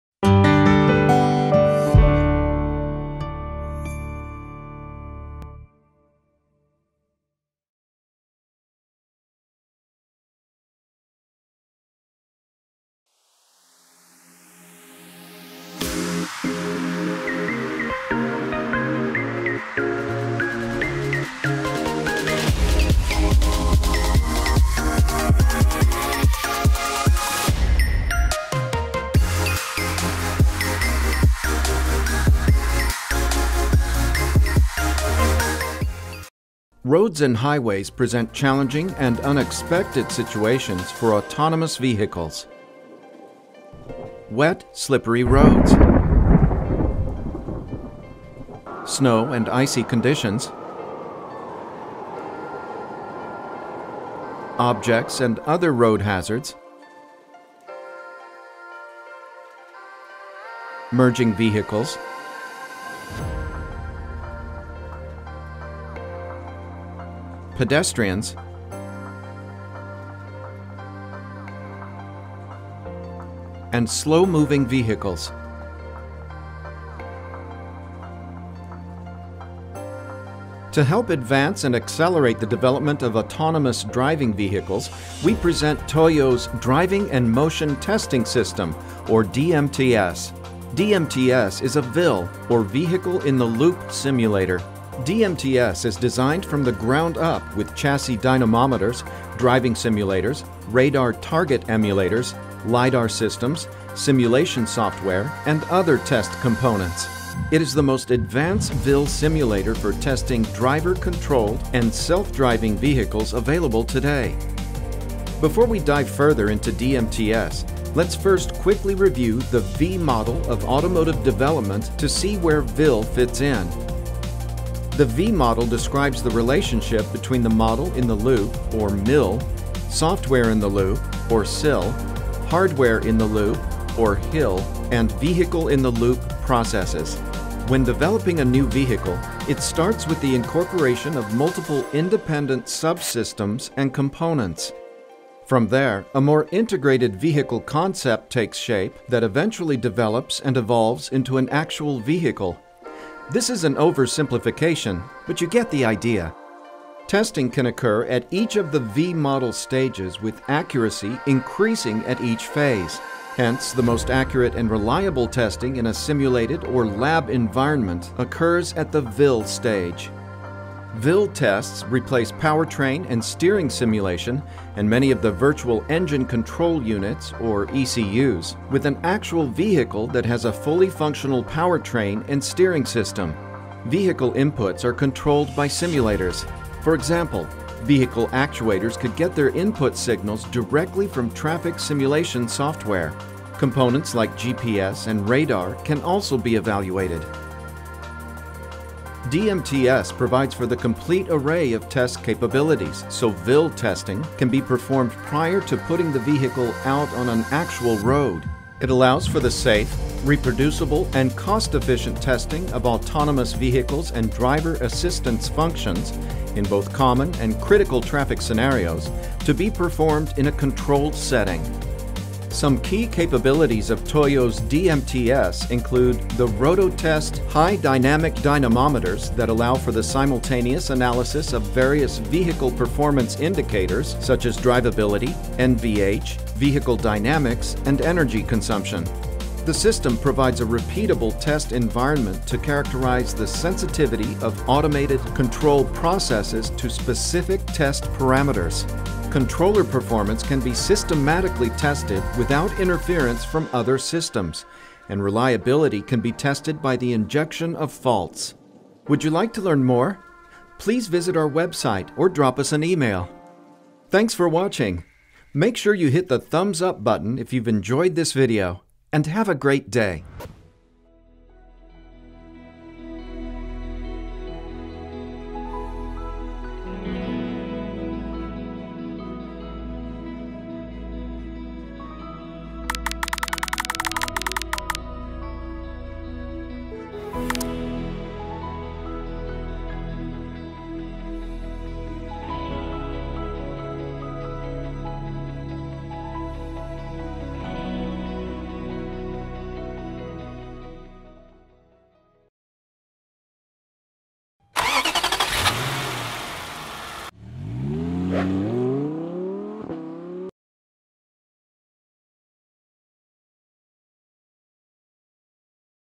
TOYO Corporation's DMTS, a Vehicle in the Loop (or VIL) Simulator (v2)
TOYO Corporation's DMTS is a Vehicle in the Loop (or VIL) Simulator --allowing you to perform testing on an actual vehicle with an actual powertrain before performing road testing.  It provides a safe, resource-efficient environment.